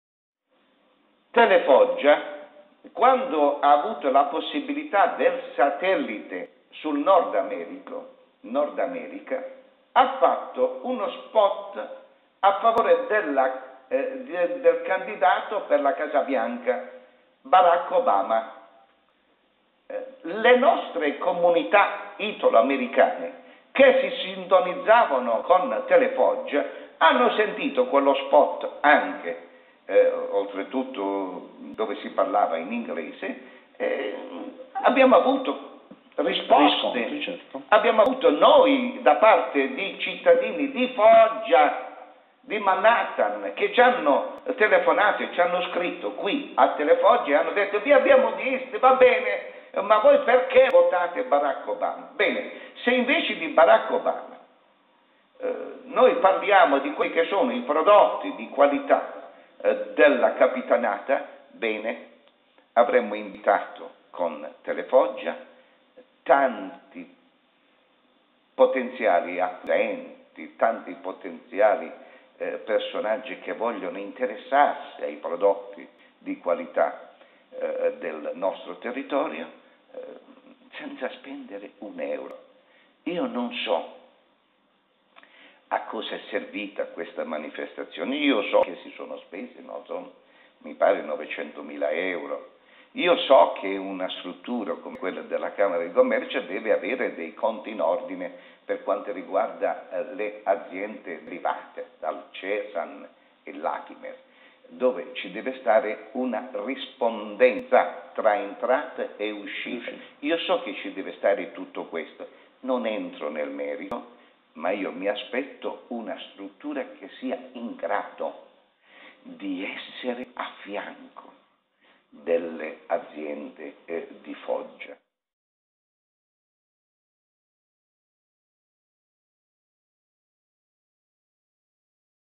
quando ha avuto la possibilità del satellite
La piccola emittente locale approda sul satellite. Ma non si deve pensare ad una piccola emittente locale e basta. Si tratta di una storica televisione locale che porta il nome della città dove è nata. Un simbolo, uno stile. Un "filone", una saga.